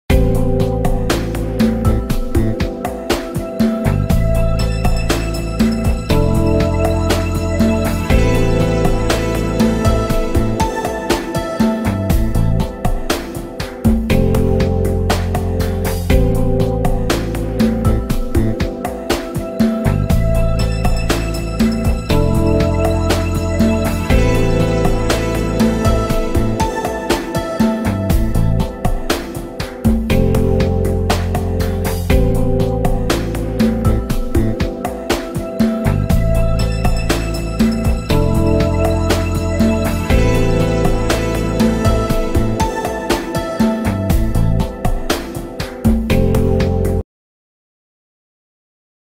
Casa - Butantã - São Paulo - SP - Ref: 576903
Casa | Butantã | São Paulo - SP | 3 dorms., 1 suíte, 2 vagas | 145 m2 área útil, 150 m2 área total